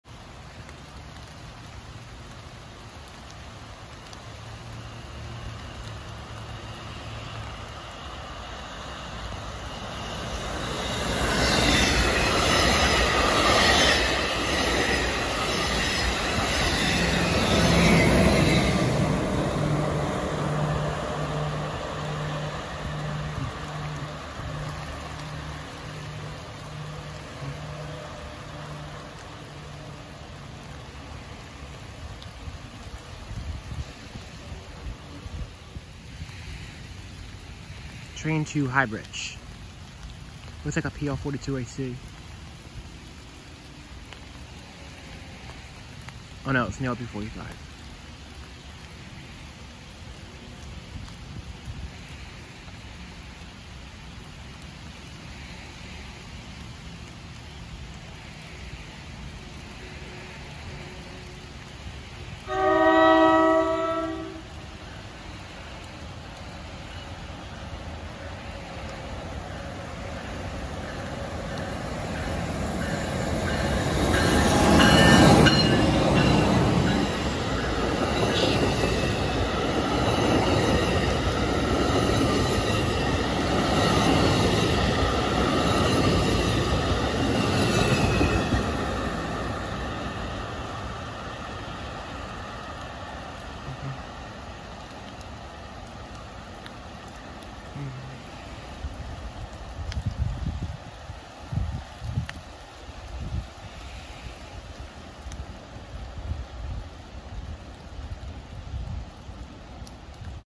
2 Multilevel trains passing through Westfield, NJ 11/11/20
Yesterday since I was bored, I decided to walk across the street from my house to see the next 2 trains.

1: NJT ALP-45DP pushing a multilevel set to Newark

2: NJT ALP-45DP pulling a multilevel set to High Bridge